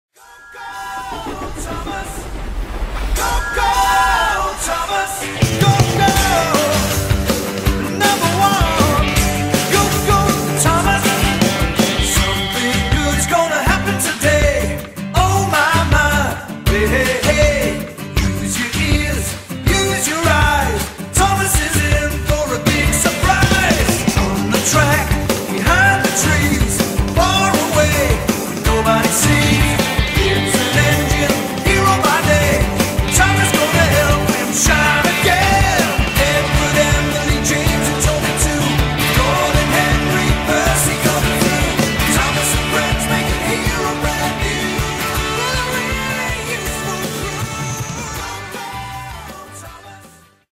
Thomas Tank Collectables Intro
This is a video to launch my new channel I will be writing reviews and using this Youtube page to show pictures of the items I review.
 TTC 2012